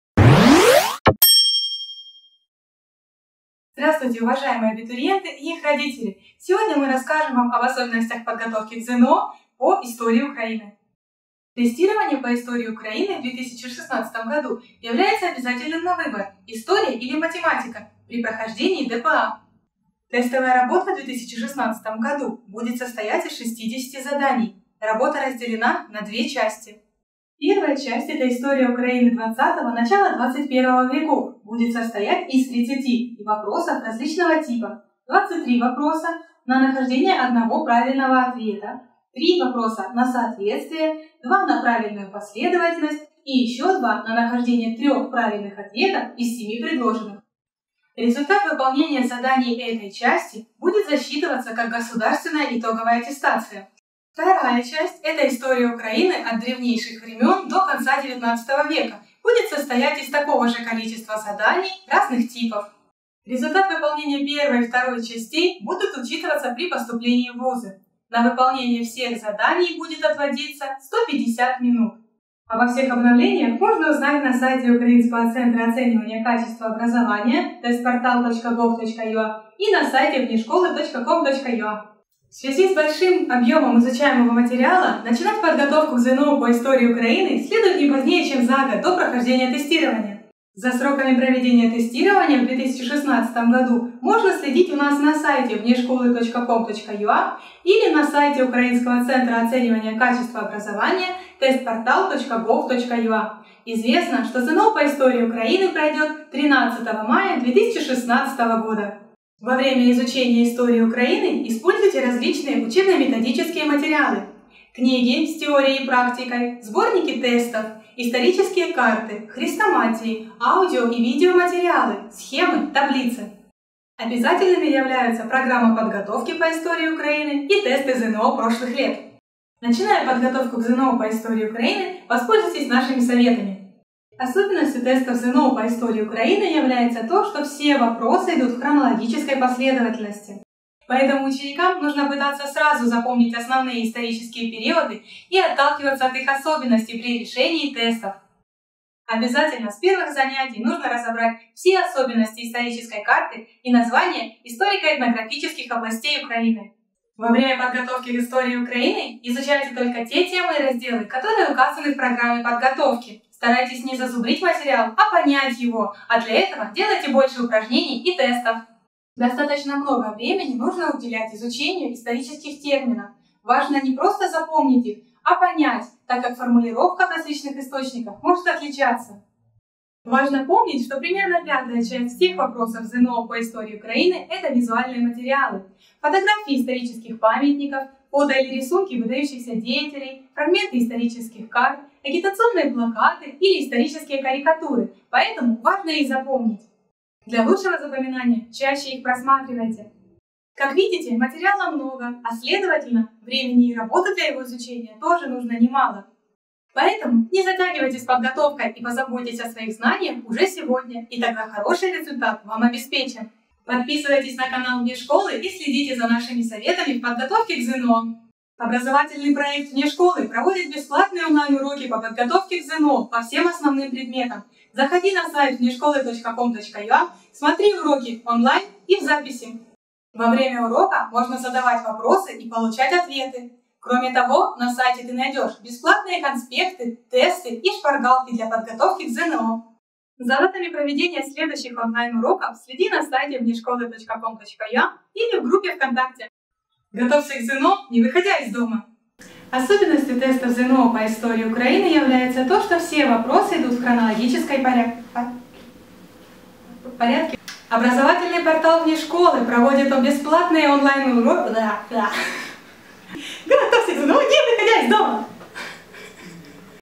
Як підготуватися до ЗНО з історії України 2015-2016 швидко та ефективно
Смотрите в ролике:
Структура и специфика тестирования ЗНО по истории Украины
Сроки подготовки к тестированию ЗНО по истории Украины
Материалы для подготовки к тестированию ЗНО по истории Украины
Особенности подготовки к тестированию ЗНО по истории Украины
Советы по подготовке к тестированию ЗНО по истории Украины